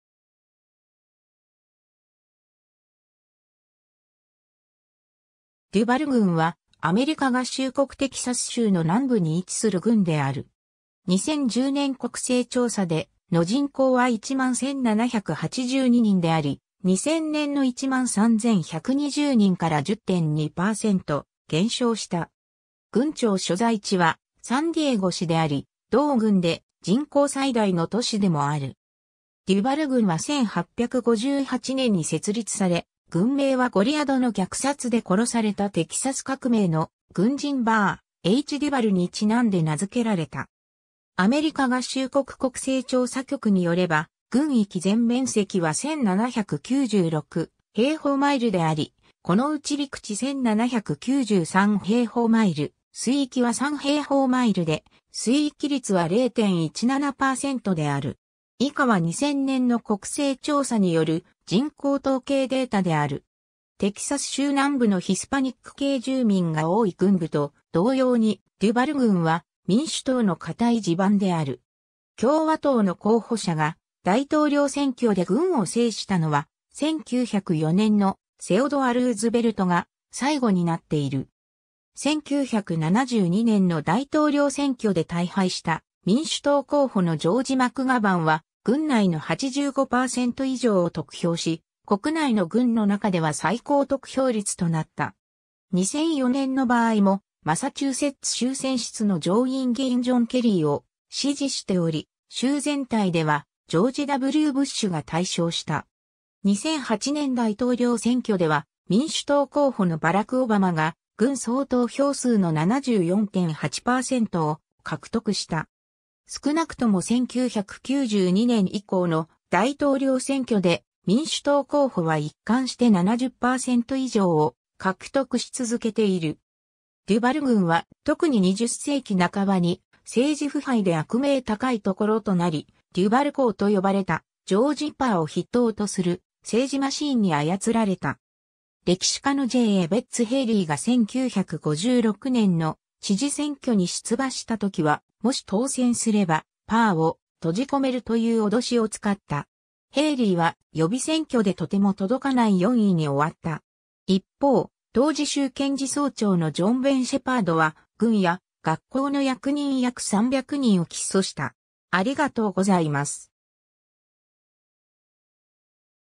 デュバル郡 (テキサス州)
デュバル郡（英: Duval County）は、アメリカ合衆国テキサス州の南部に位置する郡である。
2010年国勢調査での人口は11,782人であり、2000年の13,120人から10.2%減少した。
郡庁所在地はサンディエゴ市（人口4,488人）であり、同郡で人口最大の都市でもある。
デュバル郡は1858年に設立され、郡名はゴリアドの虐殺で殺されたテキサス革命の軍人バー・H・デュバルにちなんで名付けられた。
 アメリカ合衆国国勢調査局に拠れば、郡域全面積は1,796平方マイル (4,651.6 km2)であり、このうち陸地1,793平方マイル (4,643.8 km2)、水域は3平方マイル (7.8 km2)で水域率は0.17%である。
 テキサス州南部のヒスパニック系住民が多い郡部と同様に、デュバル郡は民主党の堅い地盤である。
共和党の候補者が大統領選挙で郡を制したのは、1904年のセオドア・ルーズベルトが最後になっている。
1972年の大統領選挙で大敗した民主党候補のジョージ・マクガヴァンは郡内の85%以上を得票し、国内の郡の中では最高得票率となった。
2004年の場合も、マサチューセッツ州選出の上院議員ジョン・ケリーを支持しており、州全体ではジョージ・W・ブッシュが大勝した。
2008年大統領選挙では、民主党候補のバラク・オバマが郡総投票数の74.8%を獲得した。
少なくとも1992年以降の大統領選挙で、民主党候補は一貫して70%以上を獲得し続けている。
 デュバル郡は特に20世紀半ばに政治腐敗で悪名高いところとなり、「デュバル公」と呼ばれたジョージ・パーを筆頭とする政治マシーンに操られた。
歴史家のJ・エベッツ・ヘイリーが1956年の知事選挙に出馬したときは、もし当選すれば、パーを「閉じ込める」という脅しを使った。
ヘイリーは予備選挙でとても届かない4位に終わった。
一方、当時州検事総長のジョン・ベン・シェパードは、郡や学校の役人約300人を起訴した。